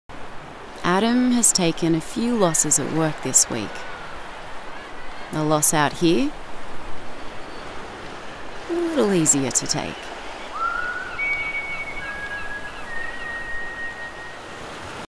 'Losses' - Life's Better Outside with Reflections Holiday Parks
You can't really lose in locations like this. And we've got stunning places all over New South Wales to holiday and let your worries drift away.

Mother Nature is at the heart of what we do. Whether you're a camper, glamper, caravaner or cabin-type of holiday-er, we've got a spot for you to enjoy the best nature has to offer.